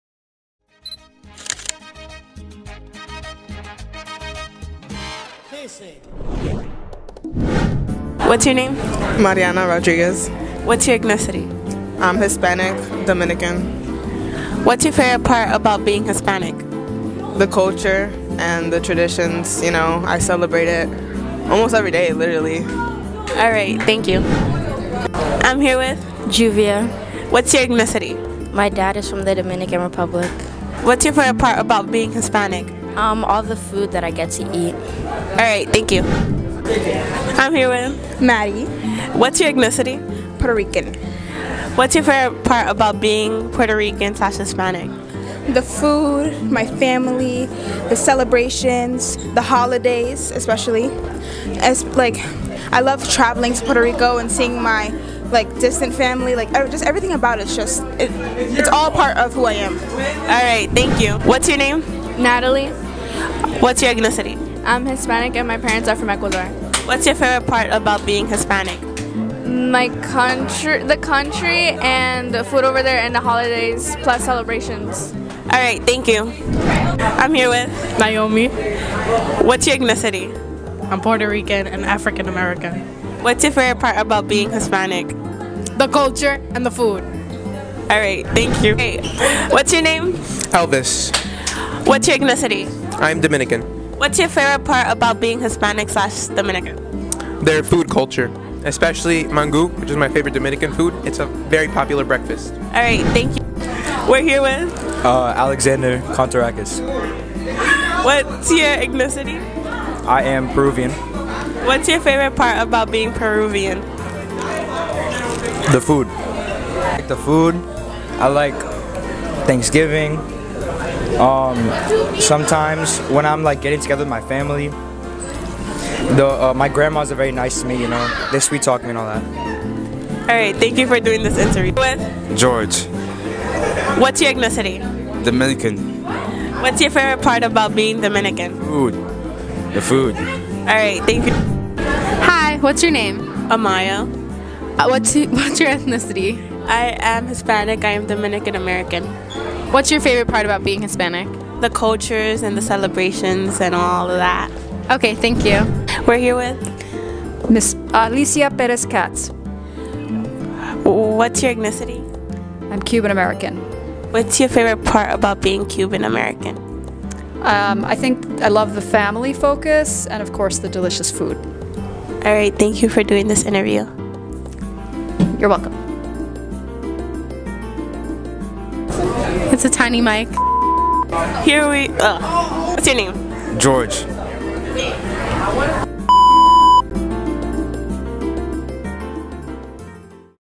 Hispanic Heritage Month @ BCCHS!
In honor of Hispanic Heritage Month at Baruch, Media Team interviewed members of our Hispanic community.

Special Thanks to:
Interviewers: Gwyneth and Amaiah
Editor: Ritika
The interviewees: Mariana, Juvia, Maddie, Natalie, Naomi, Elvis, Alexander, George, Amaiah, and Ms. Perez